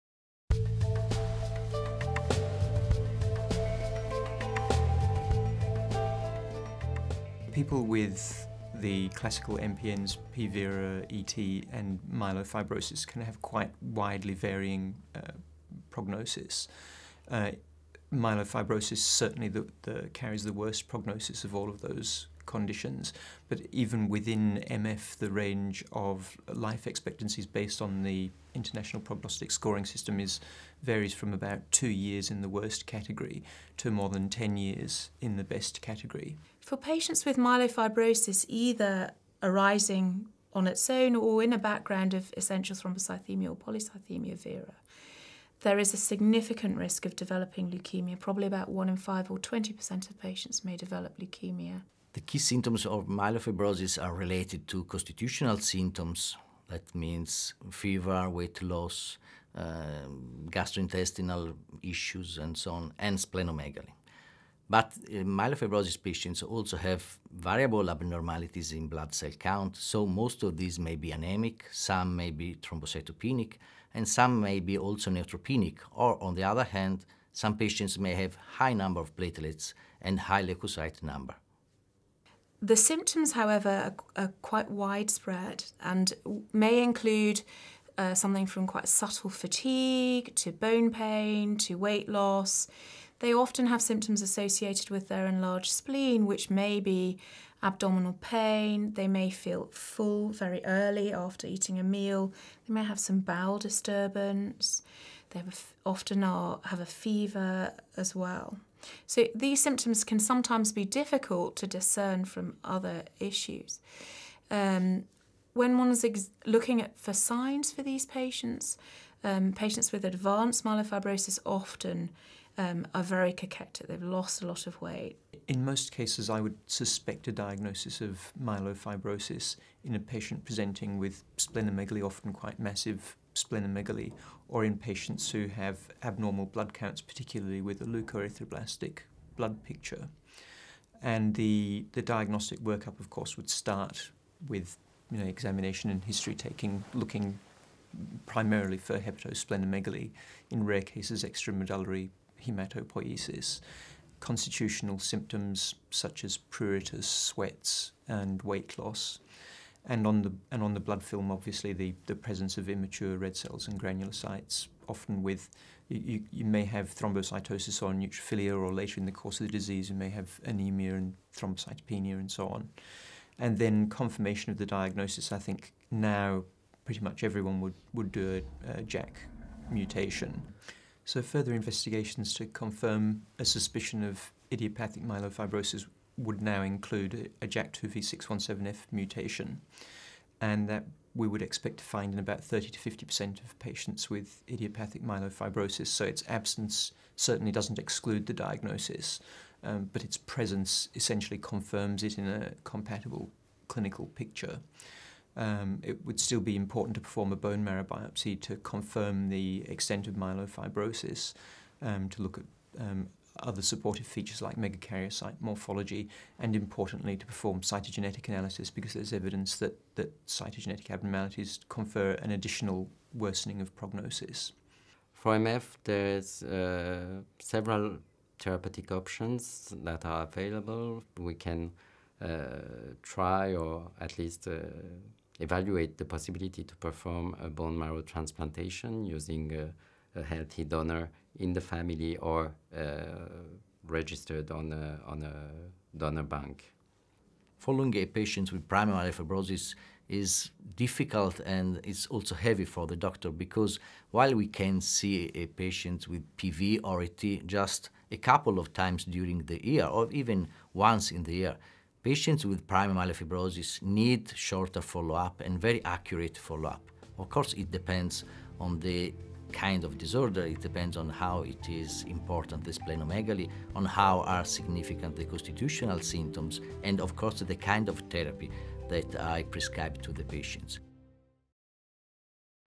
An Overview of Myelofibrosis
A discussion with a panel of global key thought leaders regarding Myeloproliferative Neoplasms (MPNs). To learn more about MPNs please visit Exploring MPN